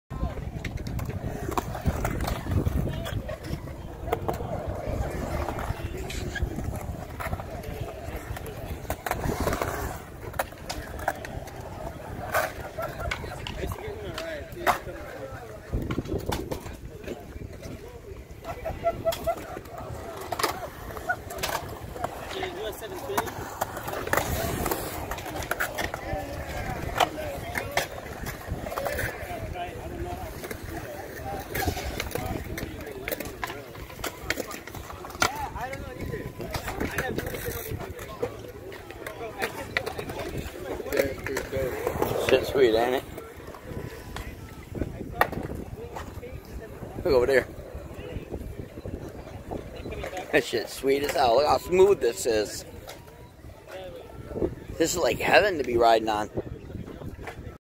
Venice Beach Skateboard Park
Stopped by to see what was going on.